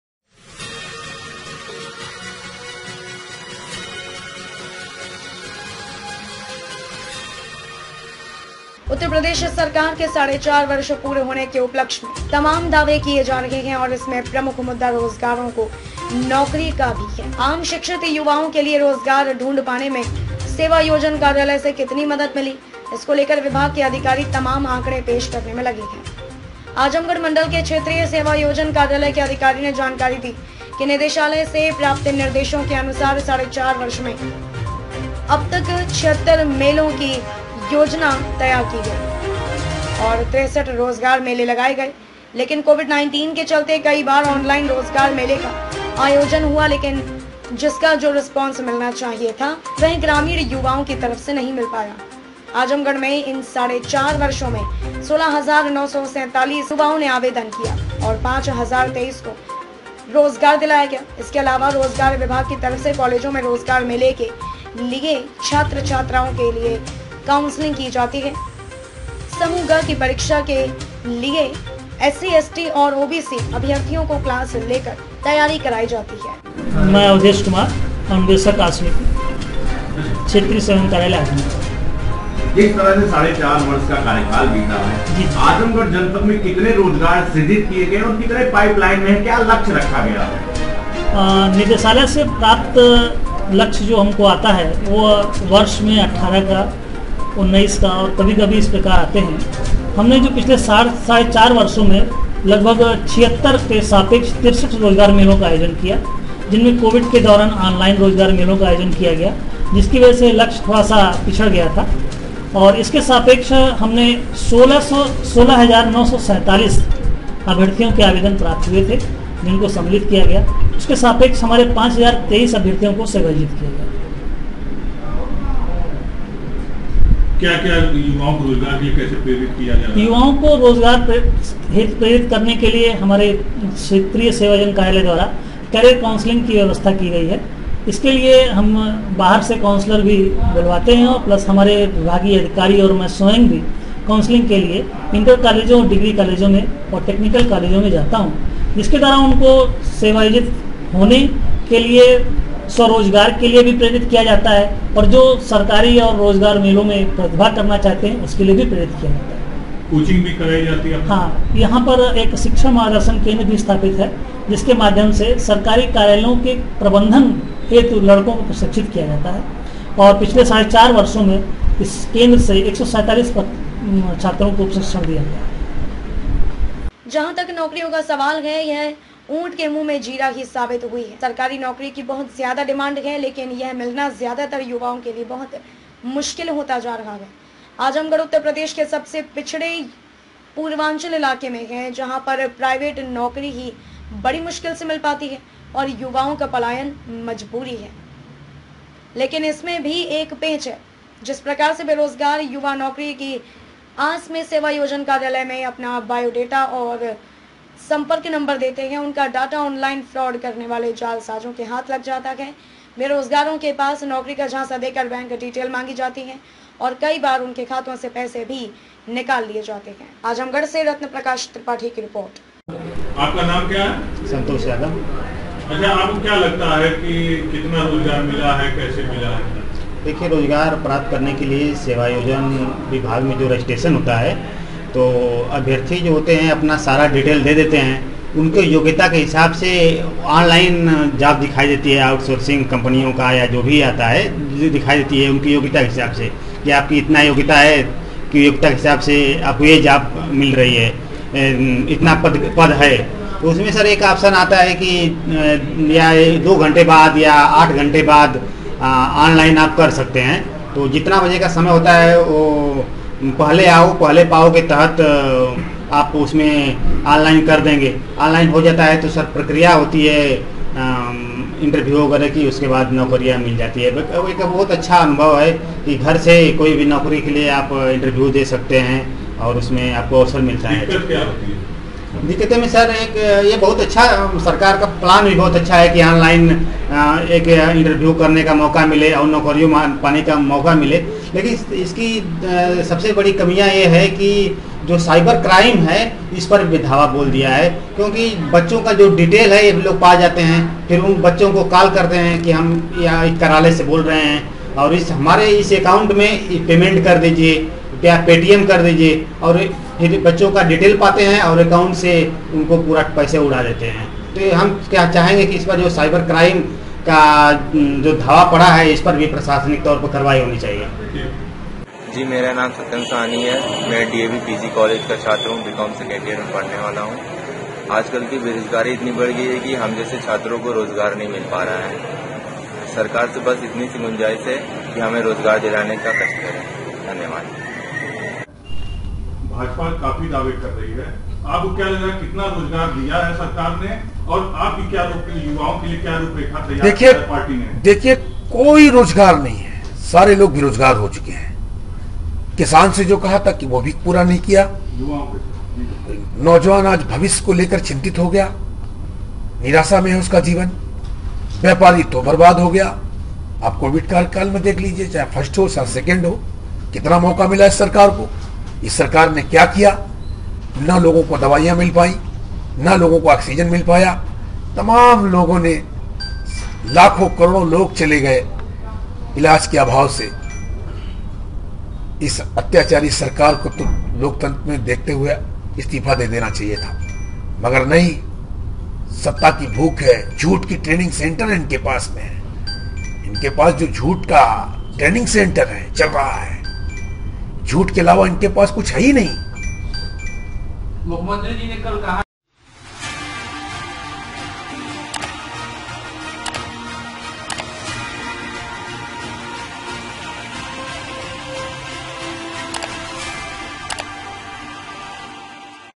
Azamgarh:- उत्तर प्रदेश सरकार की साढ़े 4 वर्ष पूरे होने पर रोजगार को लेकर उठे सवाल।।
उत्तर प्रदेश सरकार के साढ़े चार वर्ष पूरे होने के उपलक्ष्य में तमाम दावे किए जा रहे हैं और इसमें प्रमुख मुद्दा रोजगारों को नौकरी का भी है। आम शिक्षित युवाओं के लिए रोजगार ढूढ़ पाने में सेवायोजन कार्यालय से कितनी मदद मिली इसको लेकर विभाग के अधिकारी तमाम आंकड़े पेश करने में लगे हैं